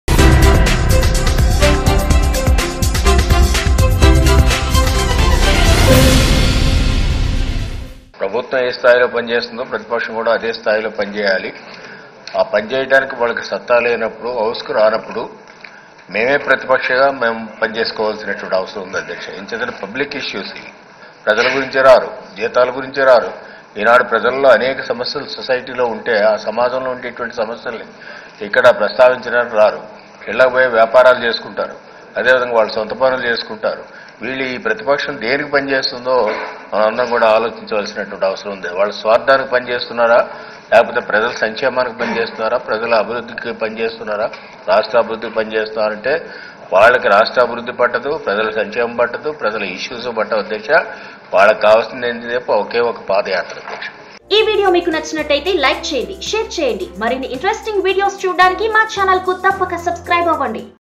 Yanamala Question Jagan's Absence from Assembly||Jagan's Padayatra||Yanamala vs Jagan
Minister for Finance, Andhra Pradesh Sri Yanamala Ramakrishnudu in his speech delivered in assembly asked the morality of being legislators if they can't attend the assembly proceedings. He made this remarks referring to YCP legislators who are not attending proceedings for the past couple of assembly sessions.

Yanamala said state exchequer is paying huge sum of salaries and allowances to MLAs and they won't be morally entitled for the same if they don't attend their job i.e. make themselves present in the assembly. Yanamala also made funny comments on Jagan's relentless Padayatra.

The senor politician said in democracy the opposition party has a very crucial role to play and here in AP, the opposition party YCP is evading that responsibility. He charged the opposition MLAs with serious allegation that they have time to build their own businesses and multiply their business turnovers but are devoid of time to attend the assembly and discuss on public issues. For opposition leader, his Padayatra comes first before public welfare.